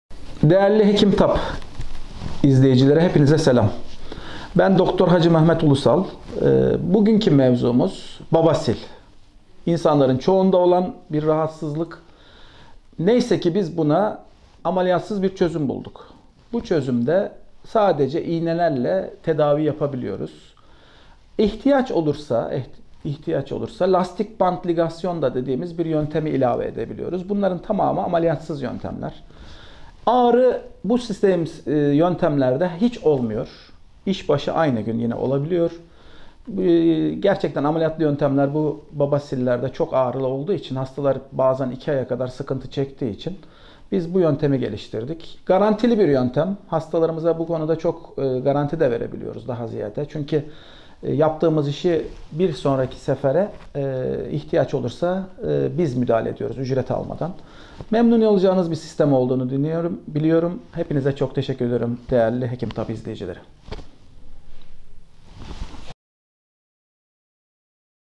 Dr. Mehmet Ulusal İxtisas: Ümumi cərrah Mövzu: Babasil xəstəliyi
Dr. Mehmet Ulusal
İxtisas: Ümumi cərrah
Mövzu: Babasil xəstəliyi
Тема: Геморрой